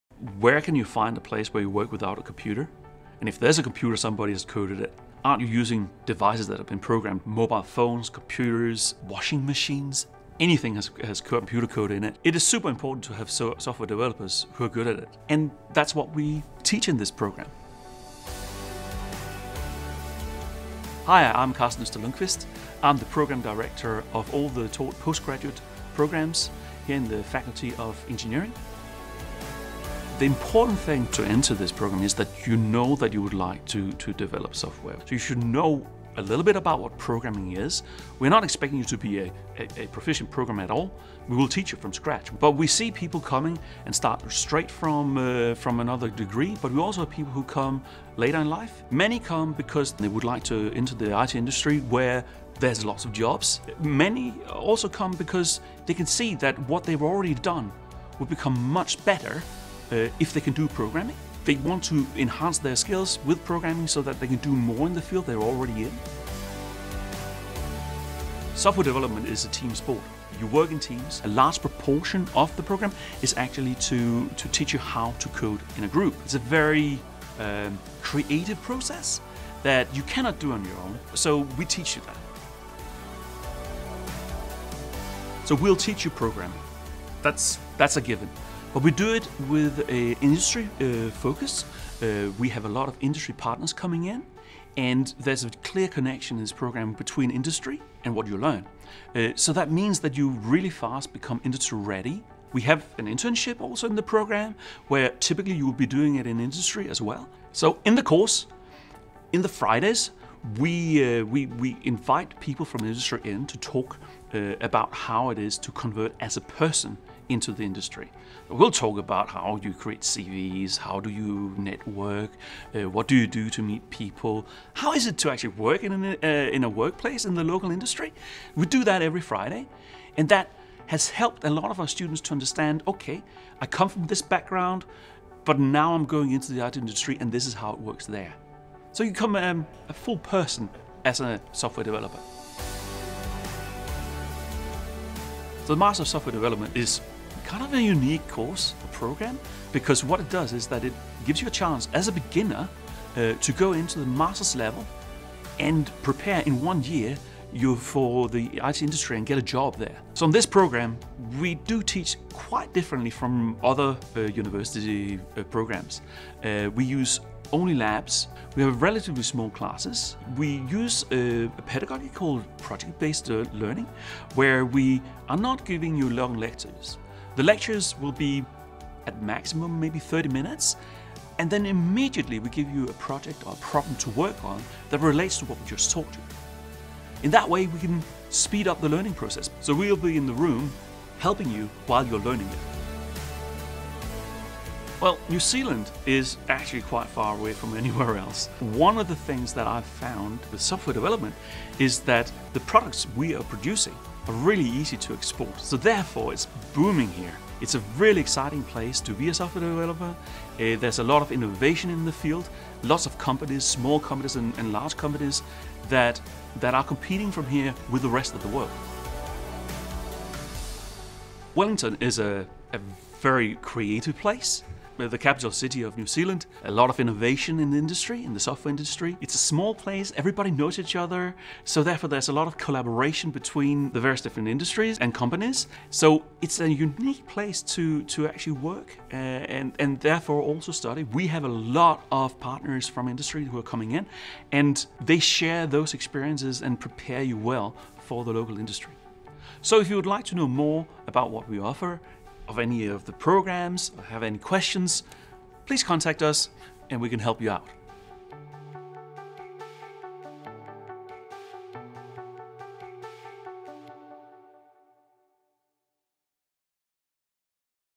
Discover the Master of Software Development
The Master of Software Development is a 12-month programme  designed to help people from non-tech backgrounds to pivot into the thriving tech industry.